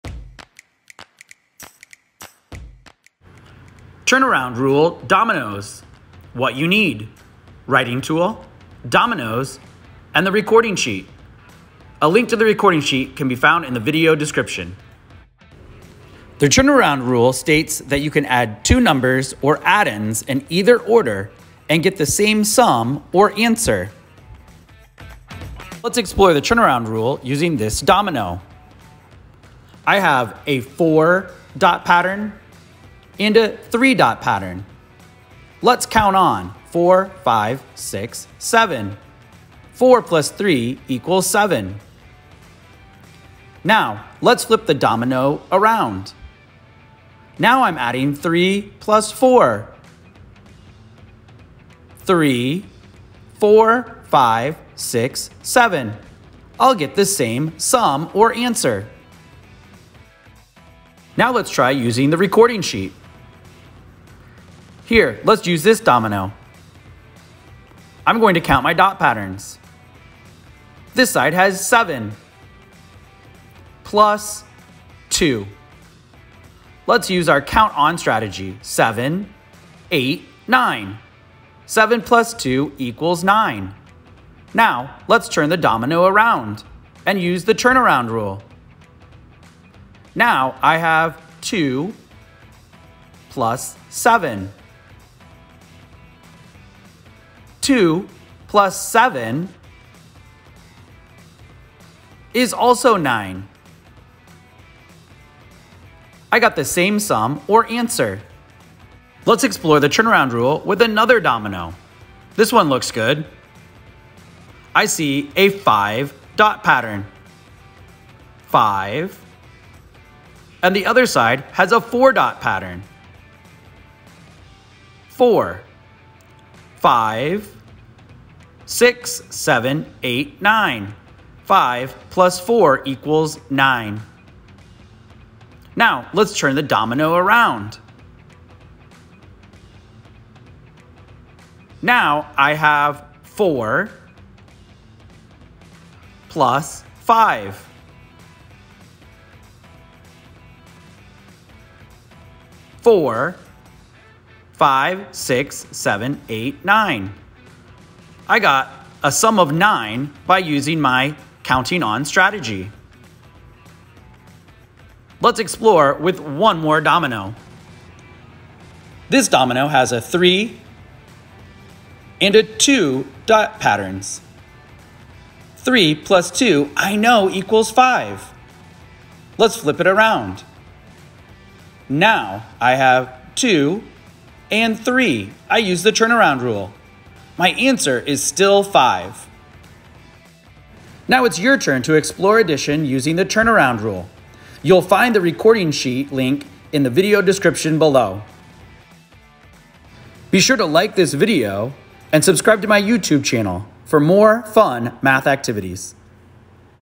Turn Around Rule Dominoes
Explore the turn around rule for addition with dominoes.  The turn around rule states that you can add two numbers or addends in either order and get the same sum or answer.